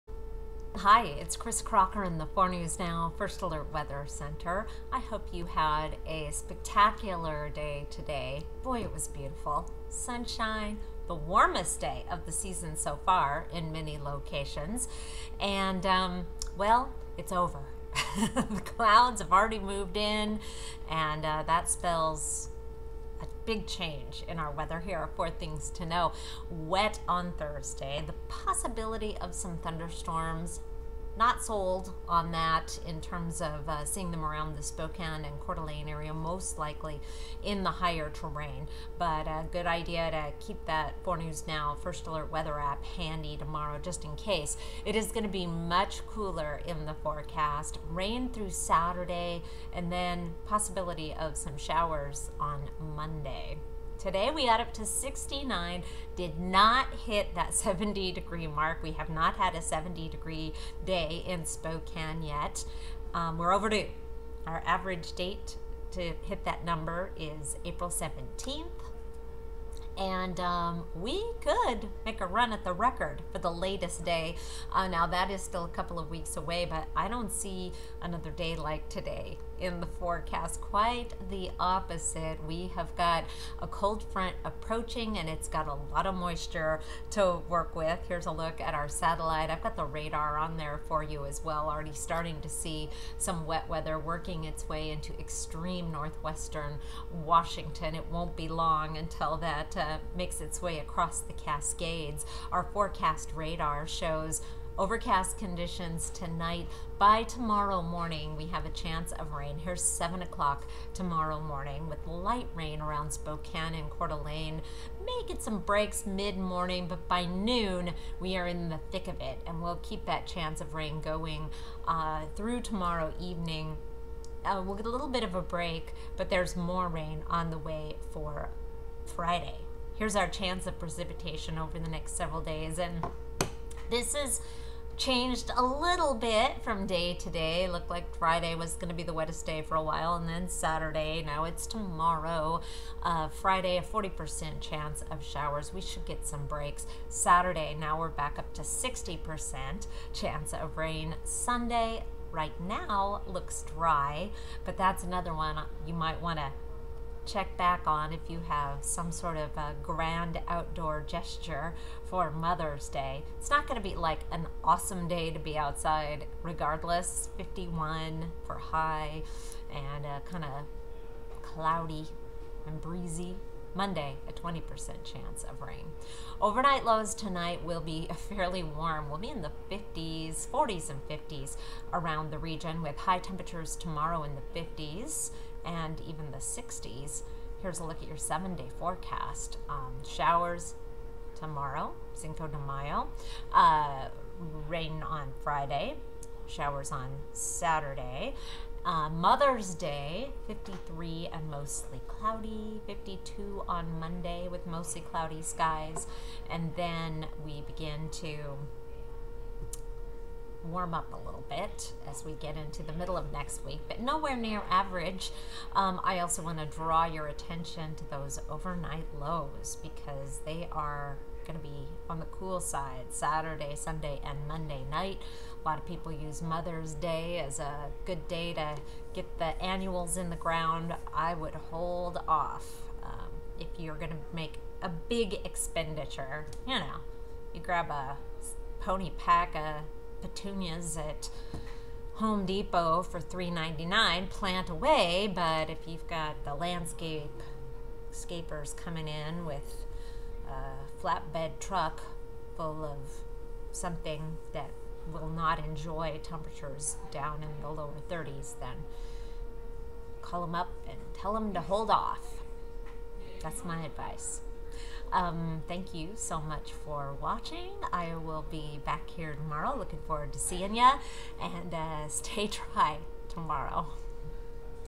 Cooler, wetter weather on the way
We ALMOST had our first 70°+ day of the season in Spokane today. We topped out at 69.1°, which was amazing. I wish I was here to tell you that it’s going to continue, but it’s not. For Thursday, expect a cooler, wetter, windier day with the possibility of some isolated thunderstorms! The best chance of rain will be right around noon, but there’s at least a slight chance all day.  High temperatures will top out in the mid 50s, which is about 10 degrees below average.

The cool weather will continue through a showery Mother’s Day Weekend and right on into next week.  GARDNERS: keep a close eye on morning lows starting Sunday and continuing into early next week.  We have the possibility for some frost, as temperatures drop into the lower to mid 30s.